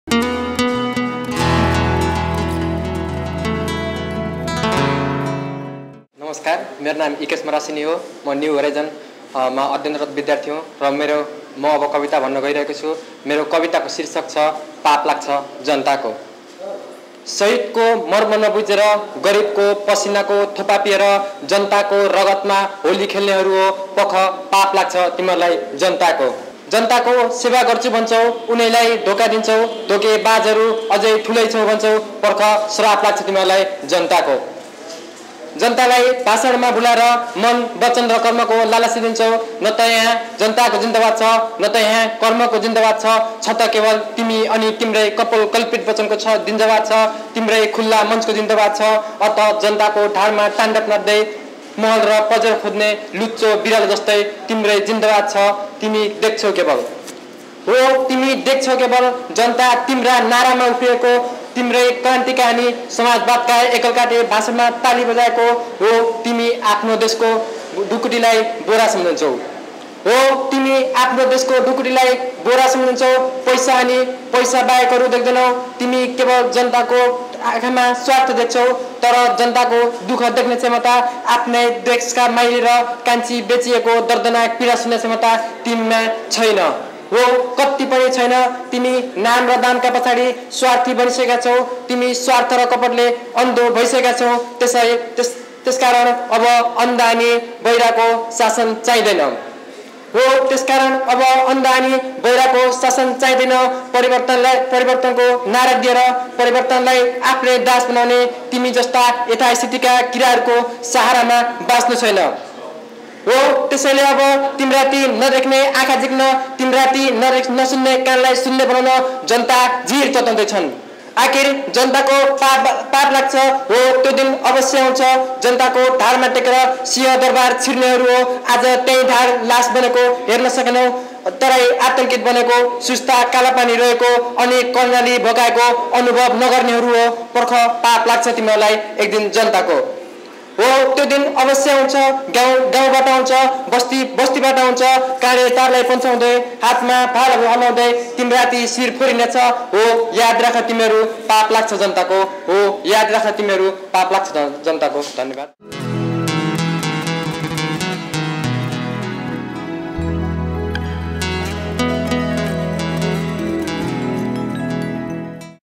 Ikesh Marasini
Audition Passed, Nepal's Top 7 Young Poets 2013
Category 3: Grade 10, 11 & 12, Nepali language

IKESH MARASINI, Grade 12
New Horizon College, Rupandehi
Hometown: Bhairahawa